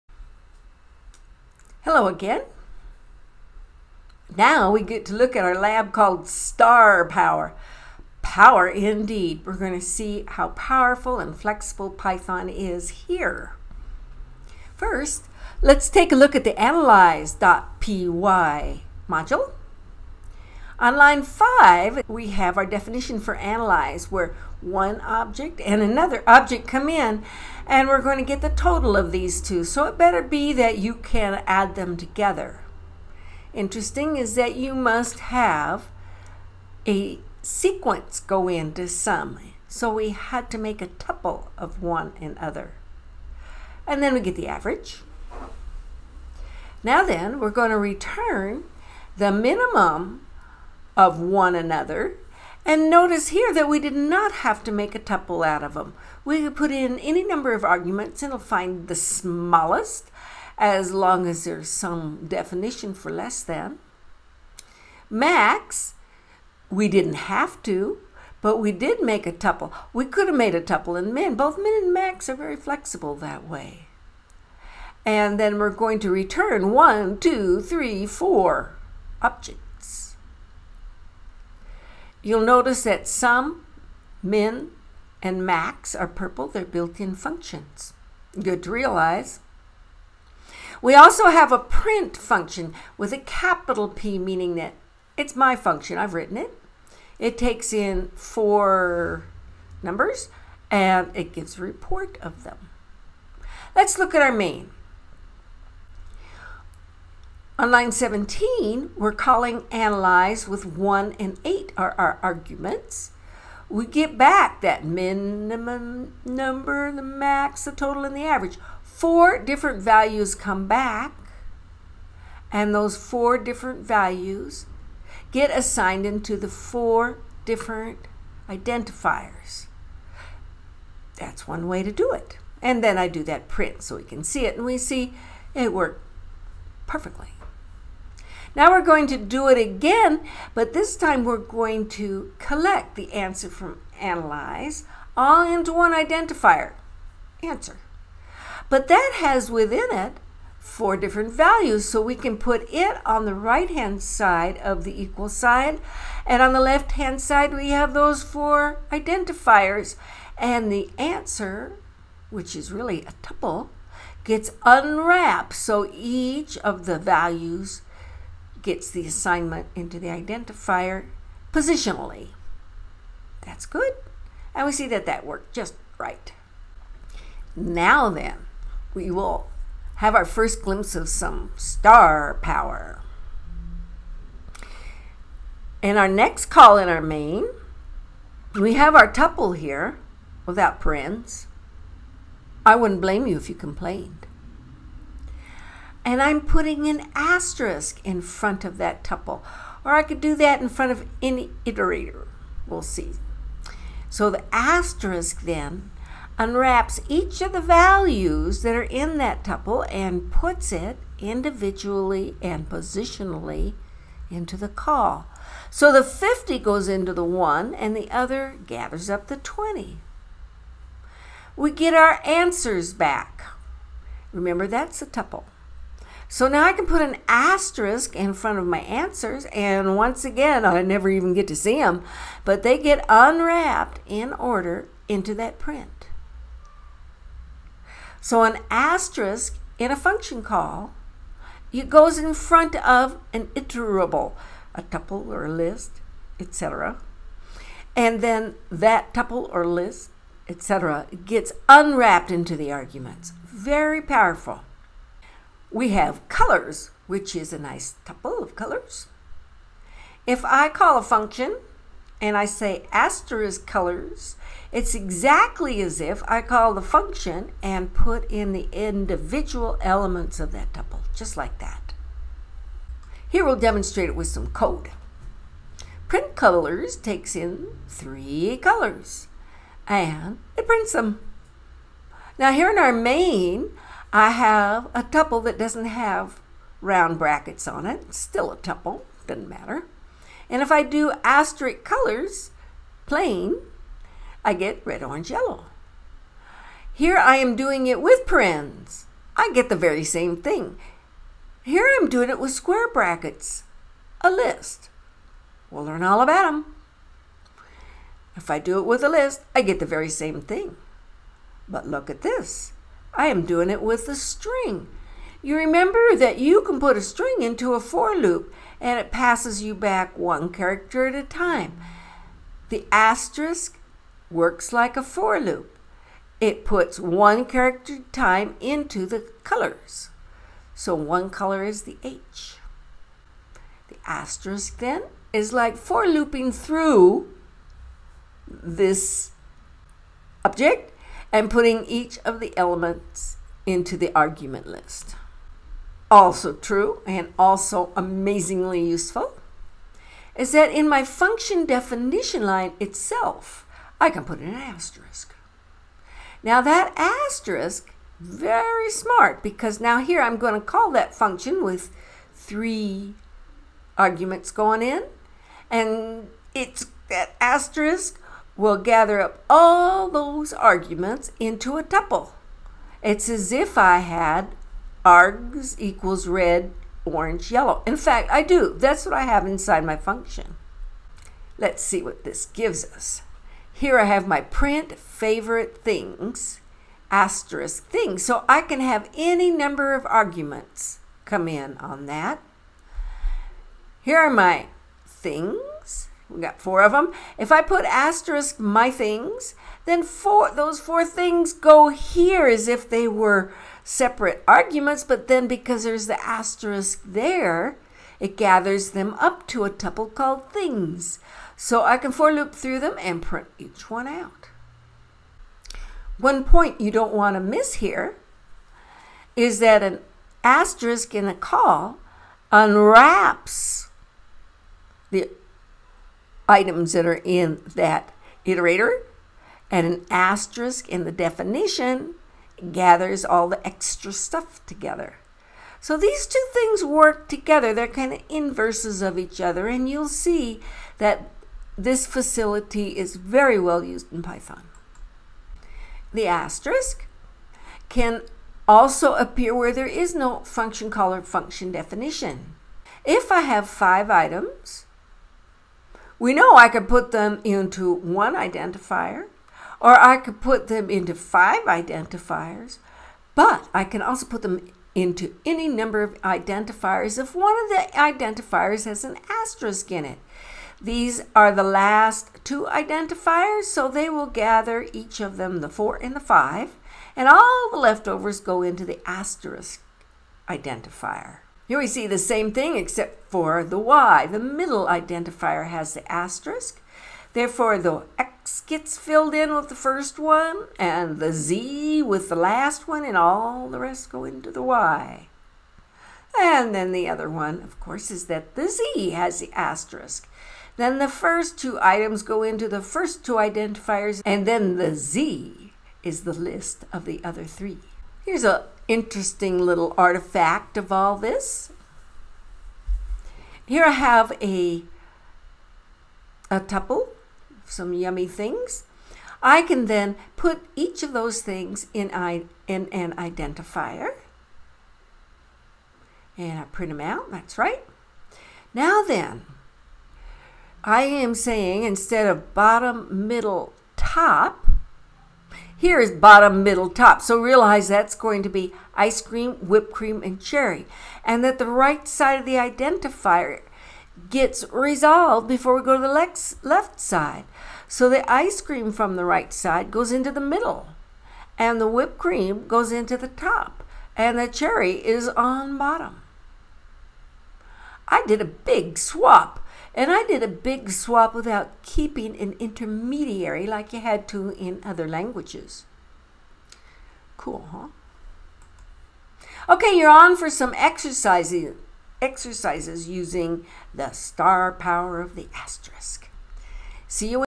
Lab060 Star Power lecture
The asterisk broadens the flexibility of function protocols and assignment from sequences.